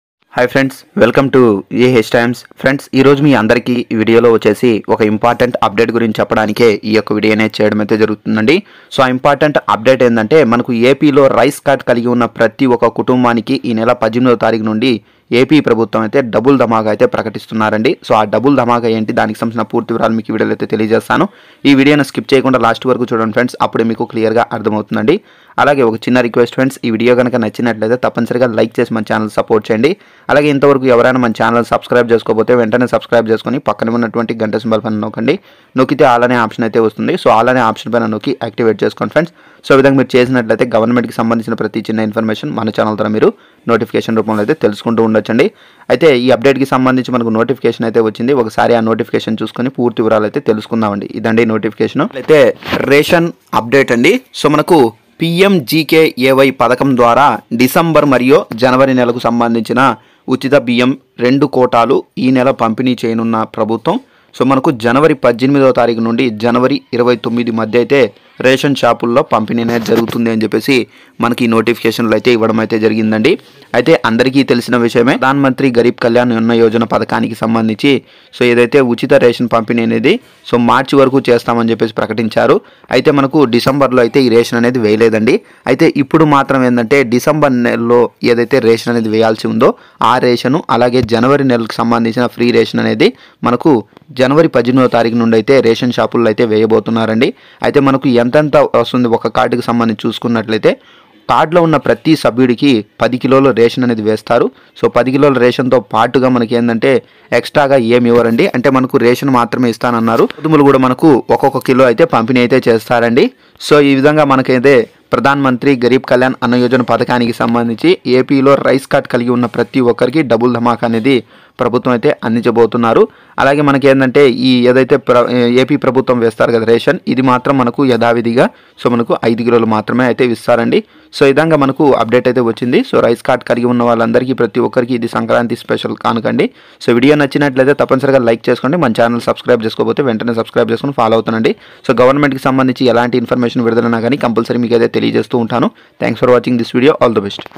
APలో రైస్ కార్డు ఉన్న వారందరికీ సంక్రాంతి కానుకగా డబుల్ థమాకా ప్రకటించిన జగన్ వెళ్లి తీసుకోండి 2022
AP Rice card Holders Good news flash updates 2022